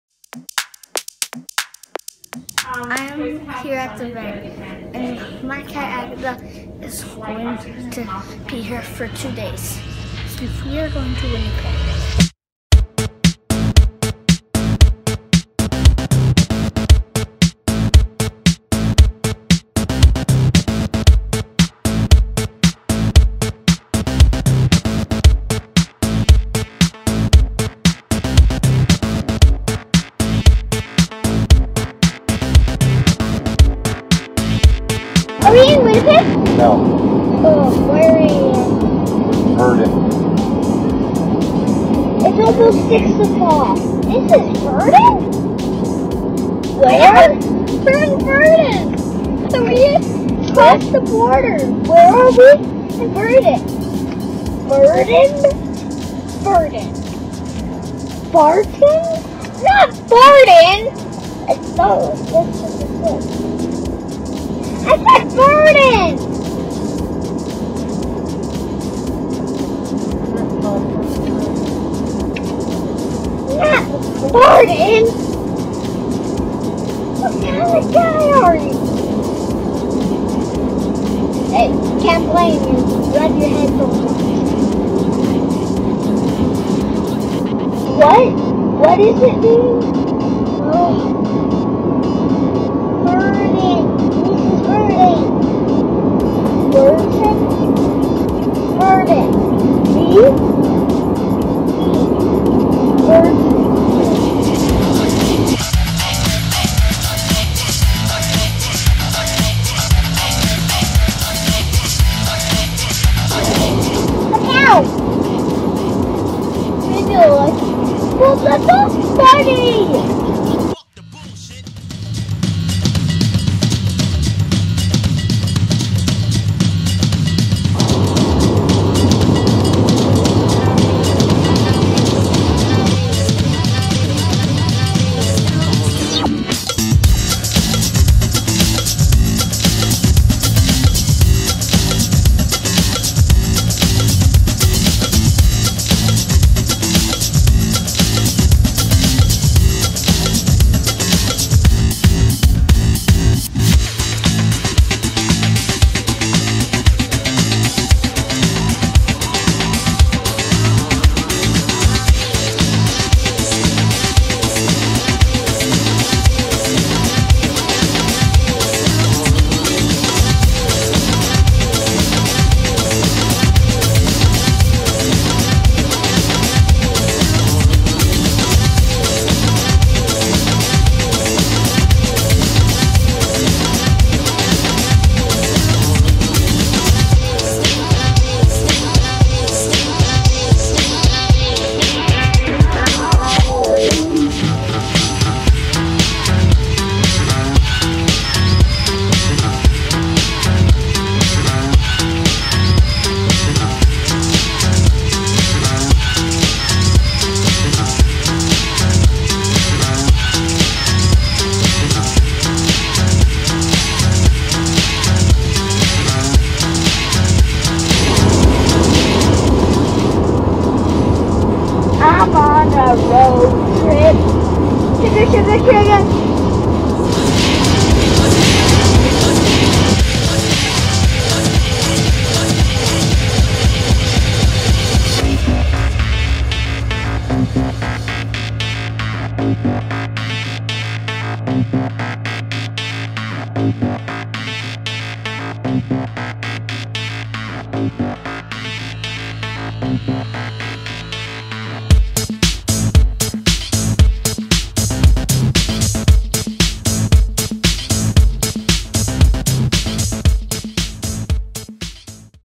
The One Where the Massey Twins go to Winnipeg
We left school early today to go to Winnipeg.  Here is the first part of the voyage!